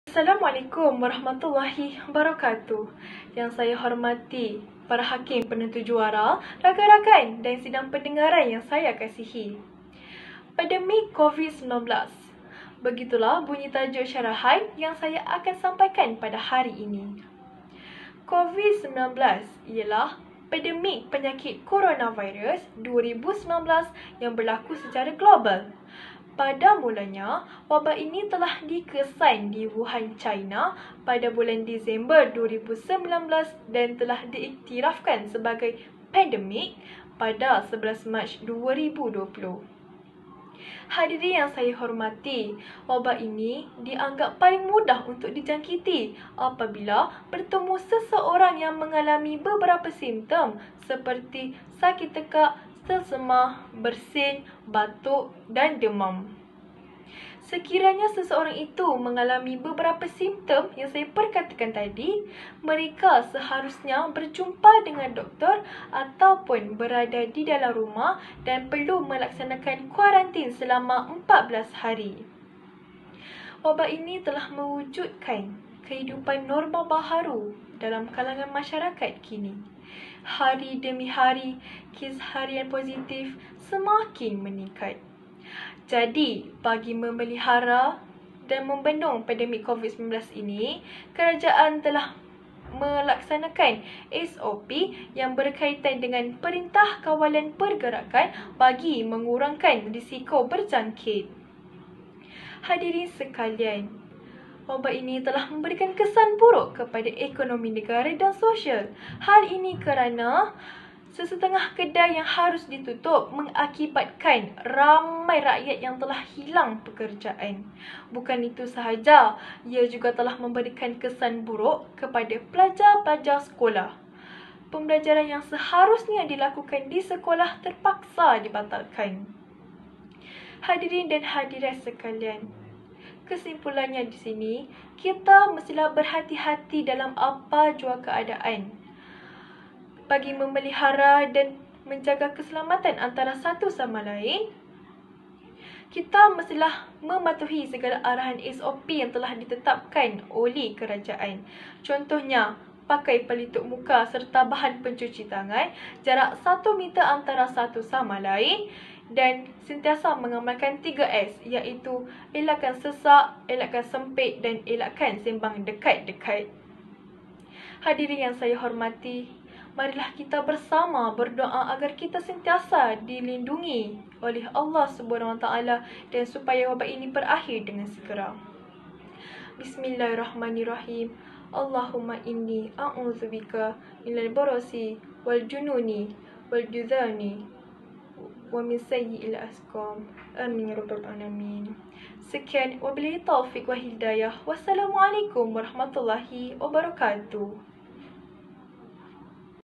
SYARAHAN: PANDEMIK COVID-19 Oleh :Iz Emylda Sorfiena bt Ilham
SYARAHAN: PANDEMIK COVID-19
Oleh :Iz Emylda Sorfiena bt Ilham
Peserta Cabaran KRS Peringkat Negeri Selangor tahun 2021